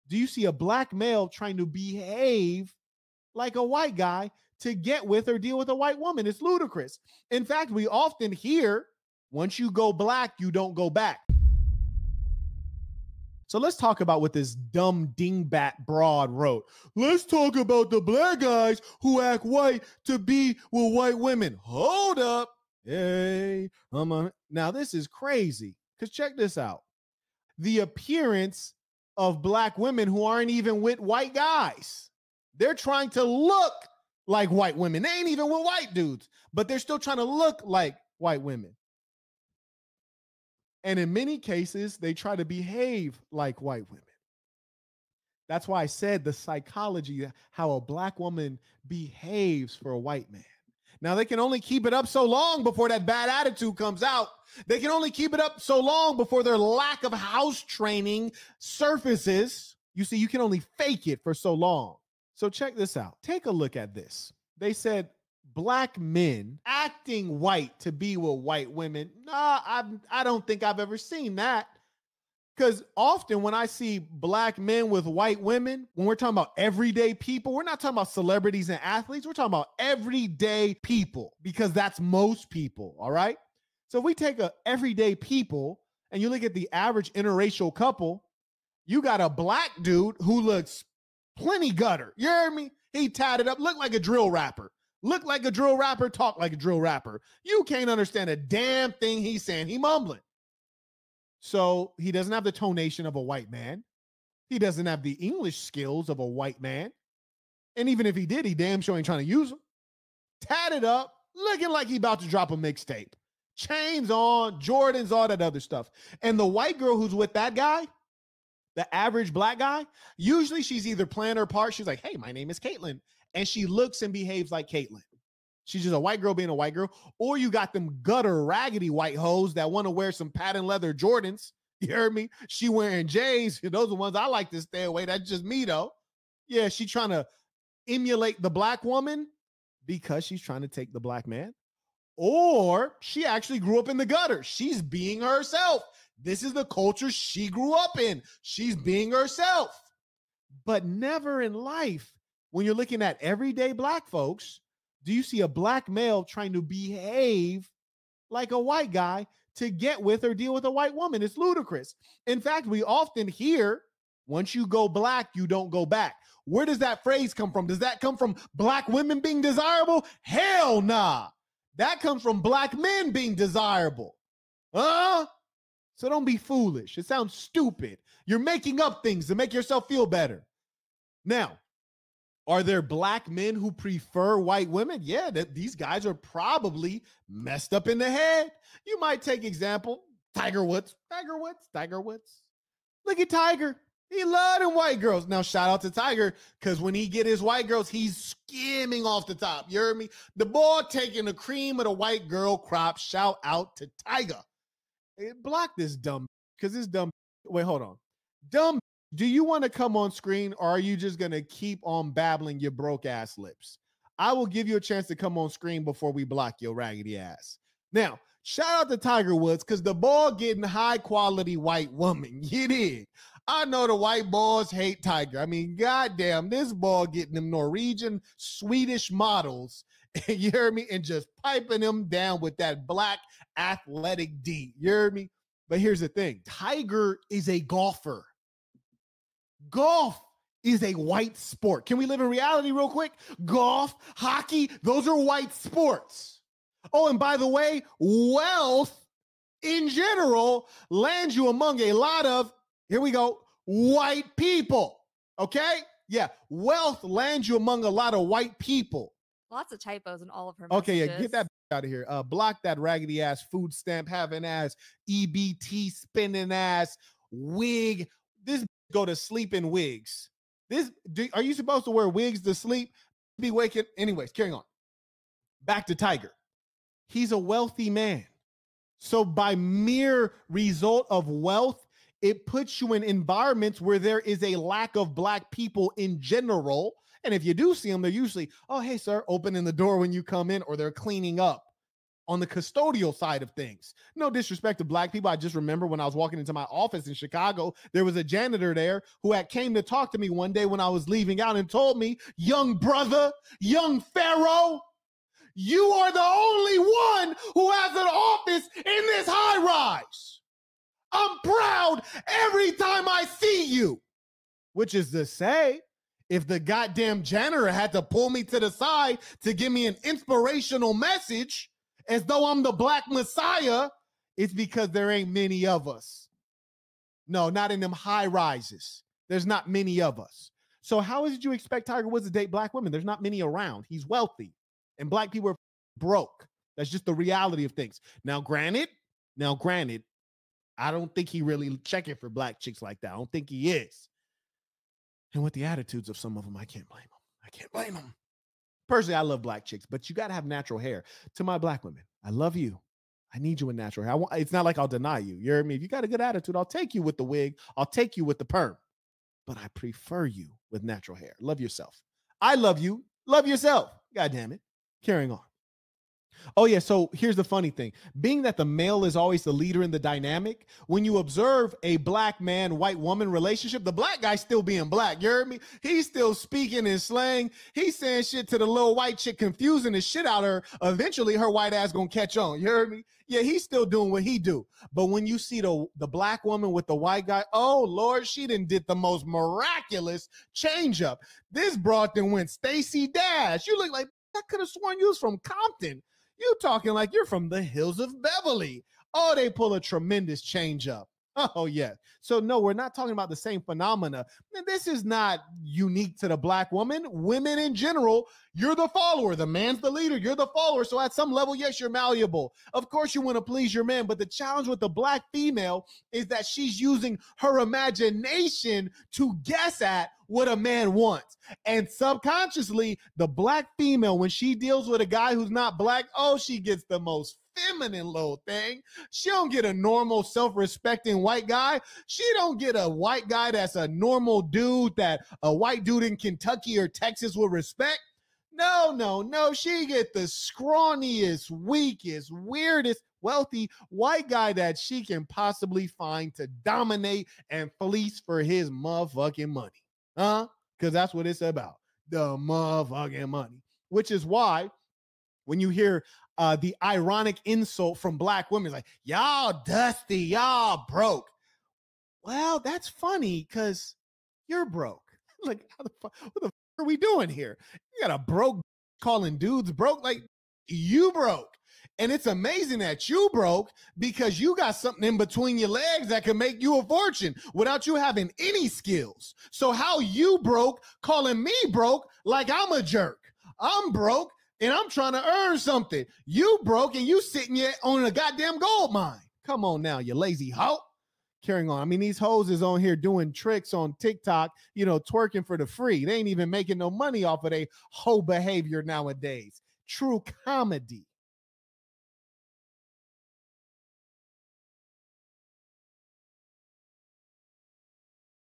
Why Black Women Transform for White Men
Marquett exposes the truth behind why black women conform to white culture when dating white men and how black men maintain their authenticity.

My Book

My recommended products

01:04 Interracial Couples: Everyday Observations
03:08 Tiger Woods and Wealth Dynamics
06:24 Black Women with White Men
07:30 Wealth and Relationships